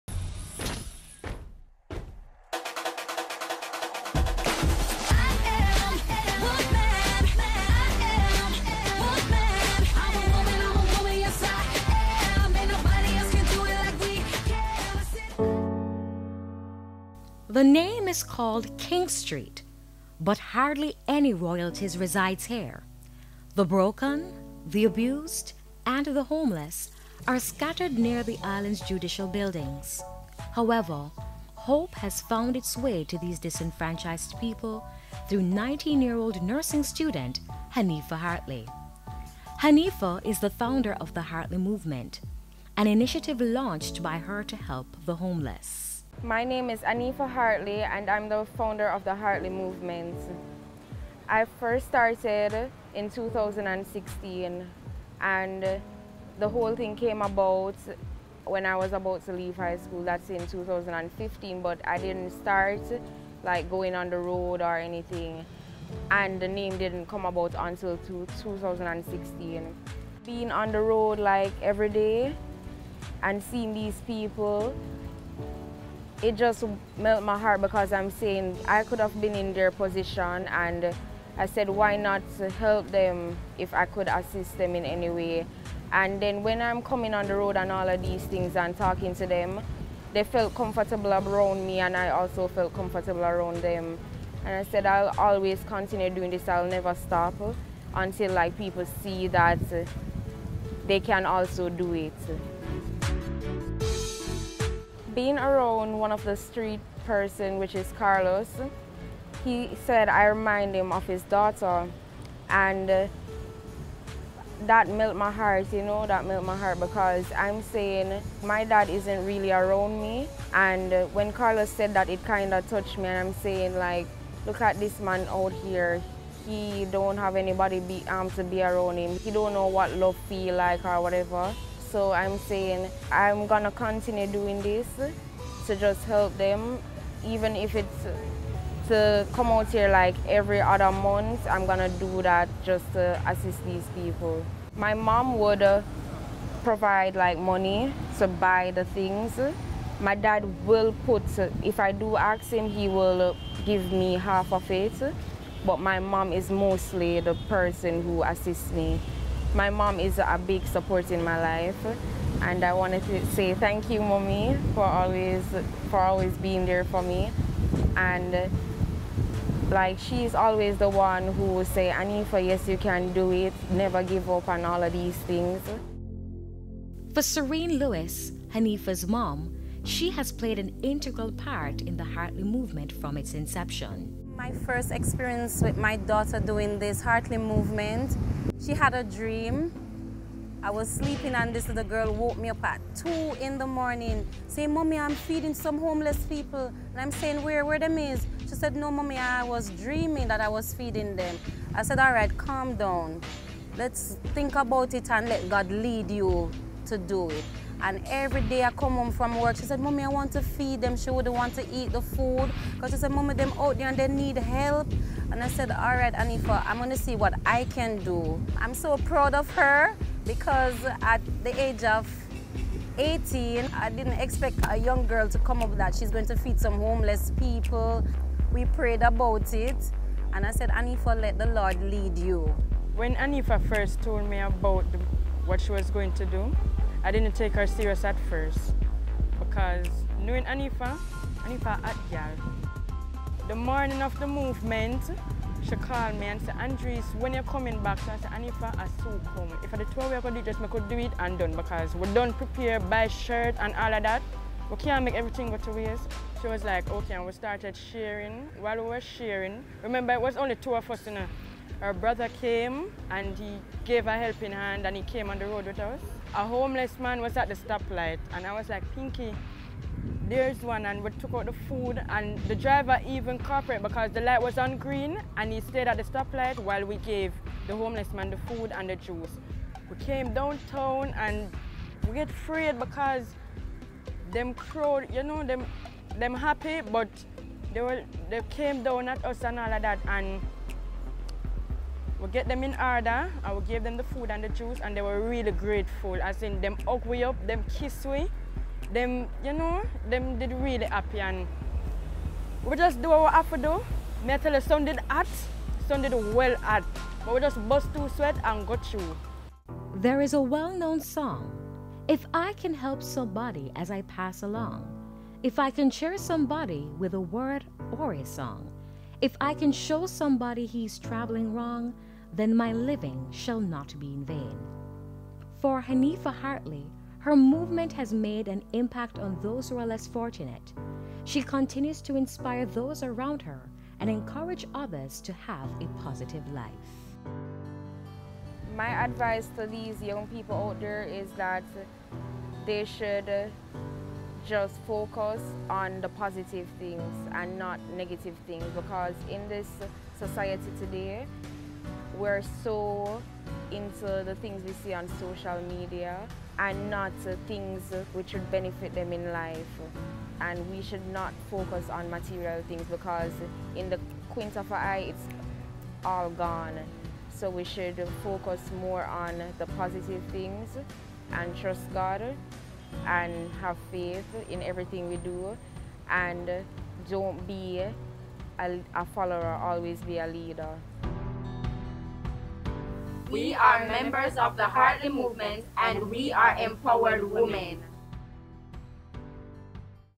Smiles Empowered Woman Feature
Empowered 19 year old Hanifah Hartley from Jamaica launched an initiative to help the homeless.